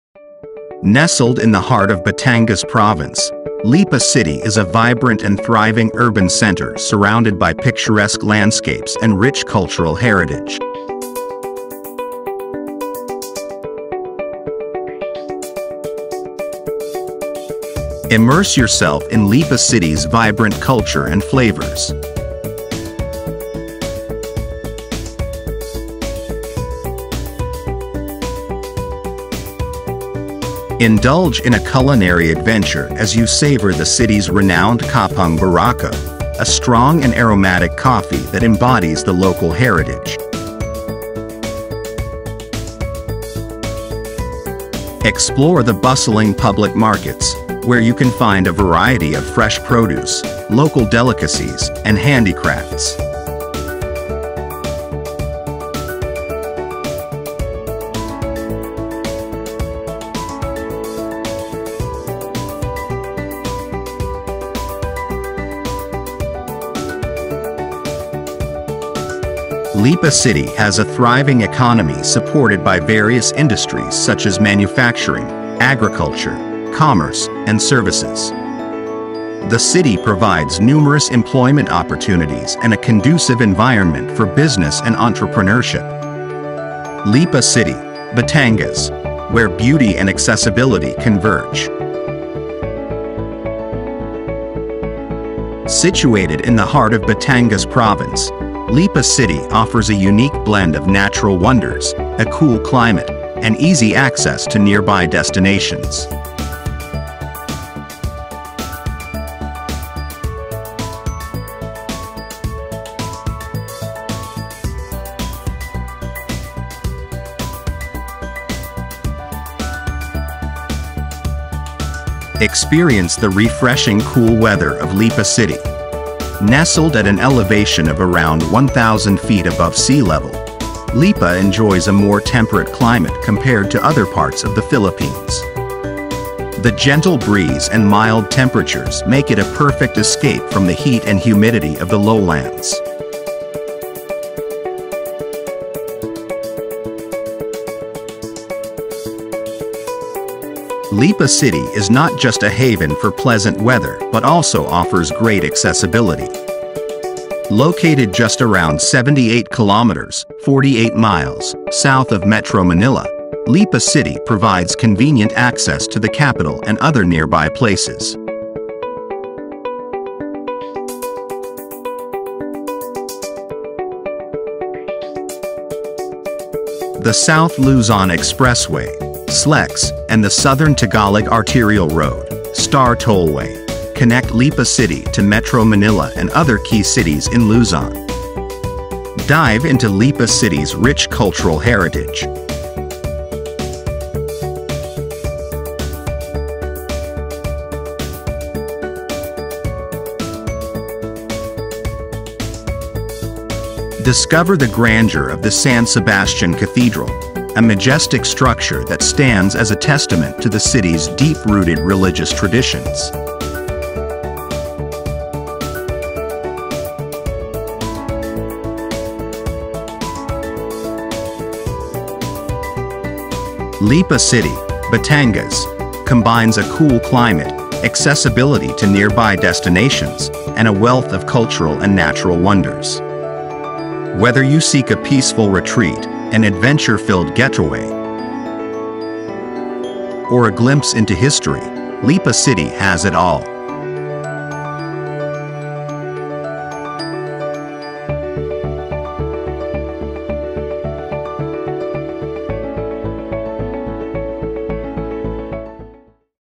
Exploring Lipa City: A Captivating Audio-Visual Journey
Join us on an immersive audio-visual adventure as we delve into the enchanting beauty and rich cultural heritage of Lipa City. Nestled in the heart of the Philippines, Lipa City is a hidden gem waiting to be discovered. Through stunning visuals and captivating storytelling, this audio-visual presentation takes you on a virtual tour of Lipa City's most iconic landmarks, breathtaking landscapes, and vibrant local communities. From the historic San Sebastian Cathedral to the picturesque Mount Malarayat, experience the allure of Lipa City like never before. Get ready to be inspired, amazed, and intrigued by the fascinating sights and sounds of this captivating destination. Whether you're a travel enthusiast, history buff, or simply curious about the world, this video is your passport to an extraordinary journey through the wonders of Lipa City. So sit back, relax, and let the magic unfold before your eyes and ears in this immersive audio-visual presentation of Lipa City.

Nestled in the heart of Batangas province, Lipa City is a vibrant and thriving urban center surrounded by picturesque landscapes and rich cultural heritage.
 Immerse yourself in Lipa City's vibrant culture and flavors. Indulge in a culinary adventure as you savor the city's renowned kapeng barako, a strong and aromatic coffee that embodies the local heritage. Explore the bustling public markets, where you can find a variety of fresh produce, local delicacies, and handicrafts.
Lipa City has a thriving economy supported by various industries such as manufacturing, agriculture, commerce, and services.
Lipa City, Batangas, where beauty and accessibility converge. Situated in the heart of Batangas province, Lipa City offers a unique blend of natural wonders, a cool climate, and easy access to nearby destinations.
Experience the refreshing cool weather of Lipa City. Nestled at an elevation of around 1,000 feet above sea level, Lipa enjoys a more temperate climate compared to other parts of the Philippines. The gentle breeze and mild temperatures make it a perfect escape from the heat and humidity of the lowlands.
Lipa City is not just a haven for pleasant weather but also offers great accessibility. Located just around 78 kilometers (48 miles) south of Metro Manila, Lipa City provides convenient access to the capital and other nearby places. The South Luzon Expressway (SLEX) and the Southern Tagalog Arterial Road (STAR Tollway) connect Lipa City to Metro Manila and other key cities in Luzon.
Dive into Lipa City's rich cultural heritage. Discover the grandeur of the San Sebastian Cathedral, a majestic structure that stands as a testament to the city's deep-rooted religious traditions.
 Lipa City, Batangas, combines a cool climate, accessibility to nearby destinations, and a wealth of cultural and natural wonders. Whether you seek a peaceful retreat, an adventure-filled getaway, or a glimpse into history, Lipa City has it all.